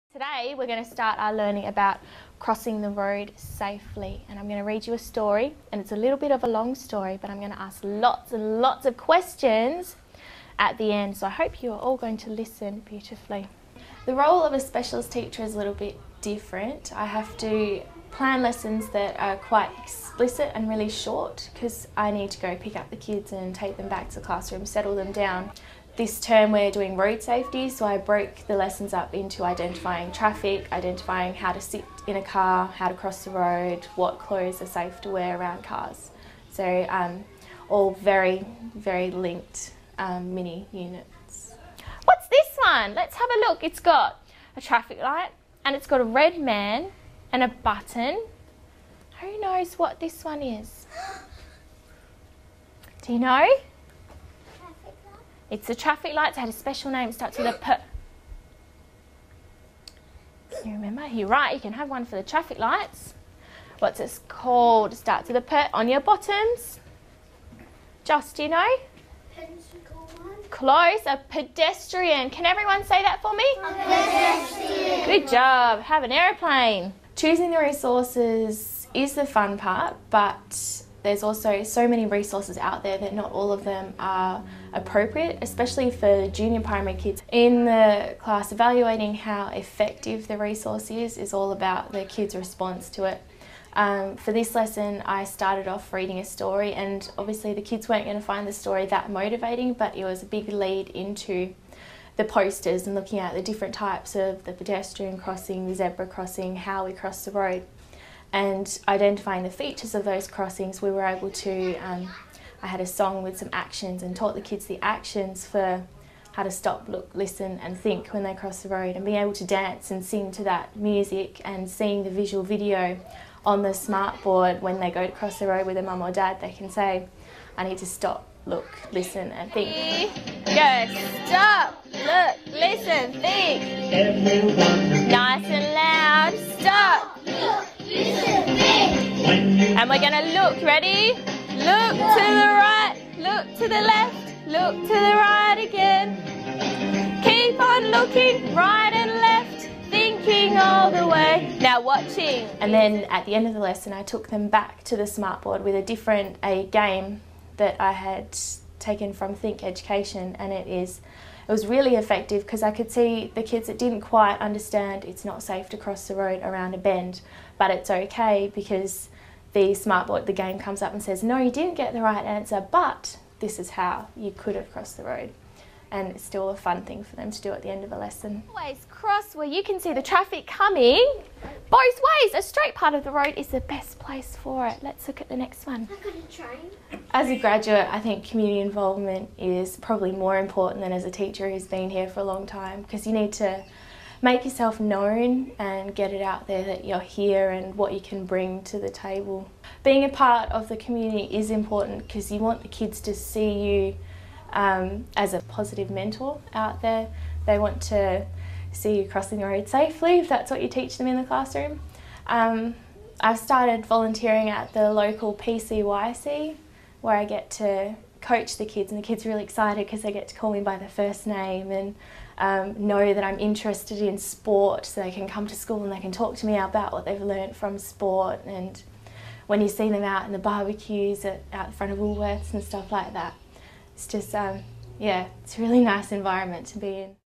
Crossing the road
Career Stage - Graduate
Main Focus area - 3.4
Secondary Focus area - 2.2
School - O’Connor Primary School

A 'specialist teacher' at O’ Connor Primary School in regional Western Australia uses ICT to engage year 1 students and support them with processing, analysing, evaluating and consolidating information about road safety. The graduate teacher begins the lesson by reading a story to the students, and then uses an ICT resource to develop student understanding of the core concepts associated with crossing the road safely. Singing, dancing and games are then used to reinforce the core concepts and as a mechanism for measuring the extent to which students have understood the lesson.